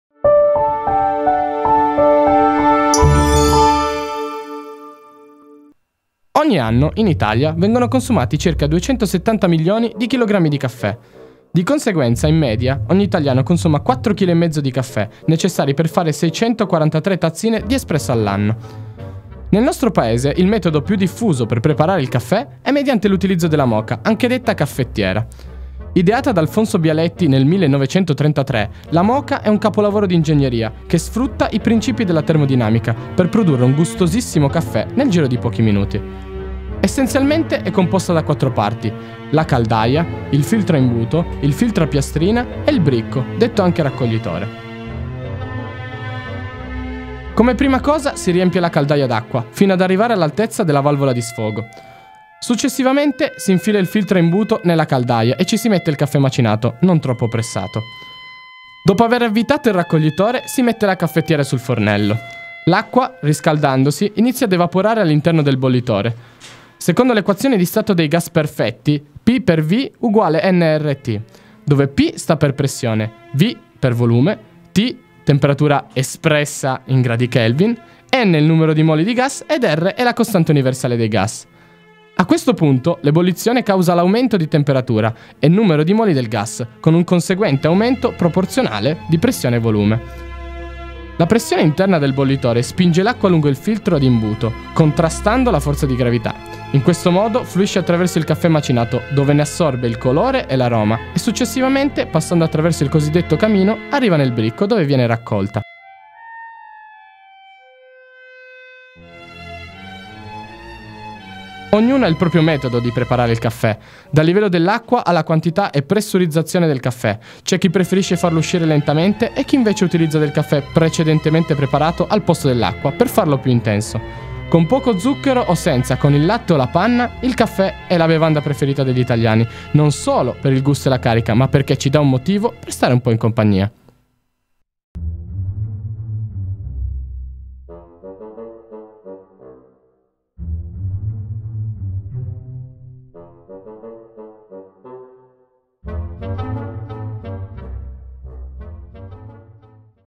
Come funziona una Moka (Caffettiera) - Scienza in Cucina
In questo breve video viene spiegato il funzionamento di una Caffettiera in modo semplice e veloce.
Se il video vi è piaciuto condividetelo.